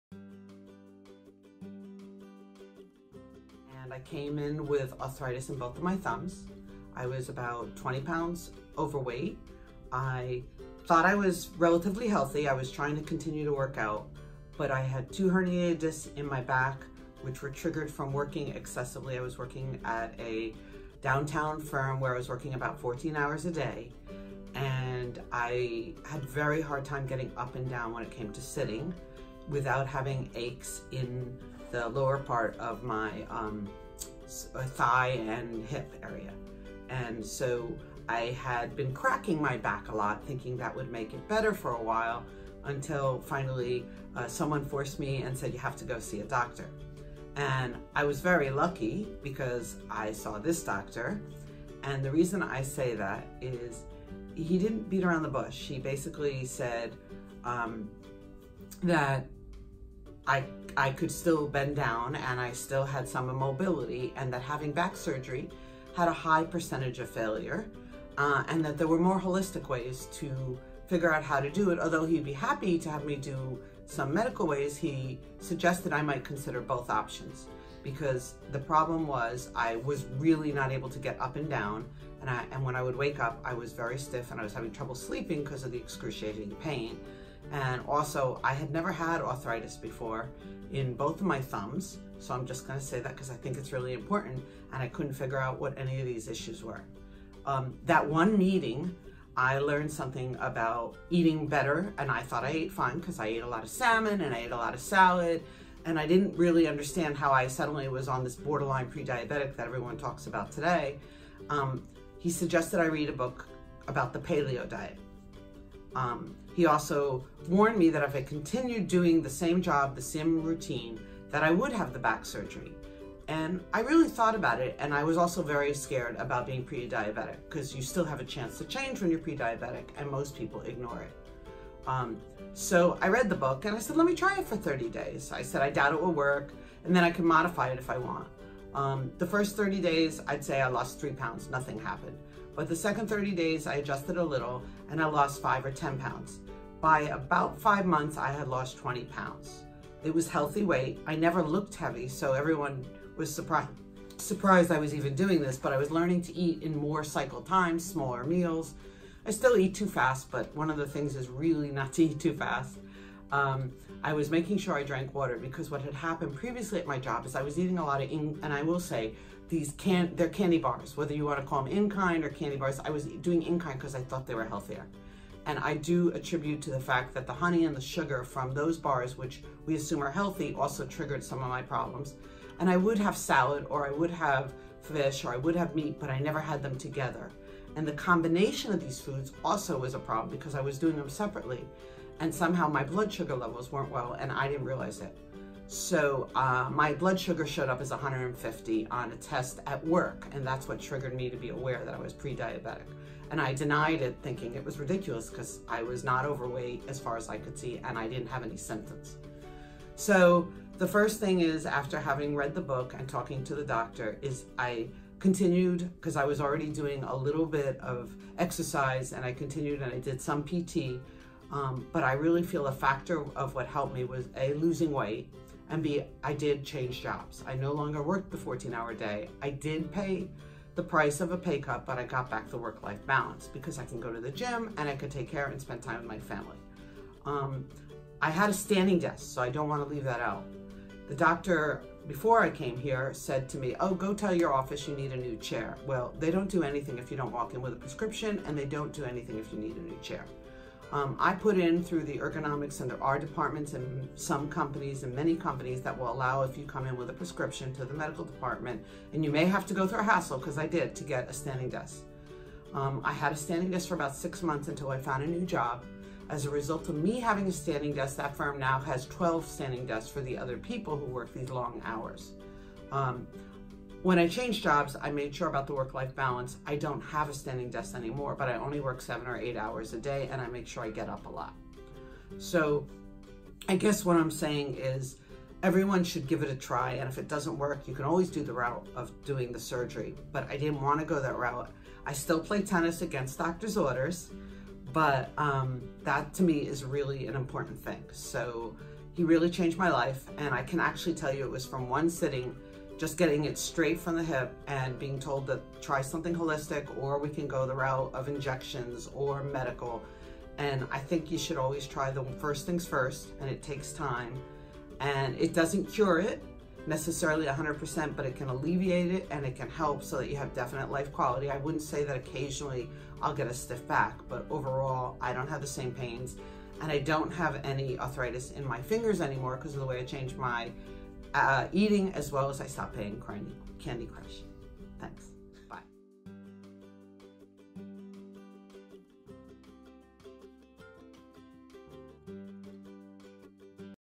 Resolution of Chronic Back Pain and Pre-Diabetes Resolved With Anti-Inflammatory Diet
Stressed, working long hours and not overweight but pre-diabetic with chronic back pain and thumb pain. After stress reduction and dietary changes, she noted weight loss, resolution of high blood sugar and a resolution of her multiple pain complaints.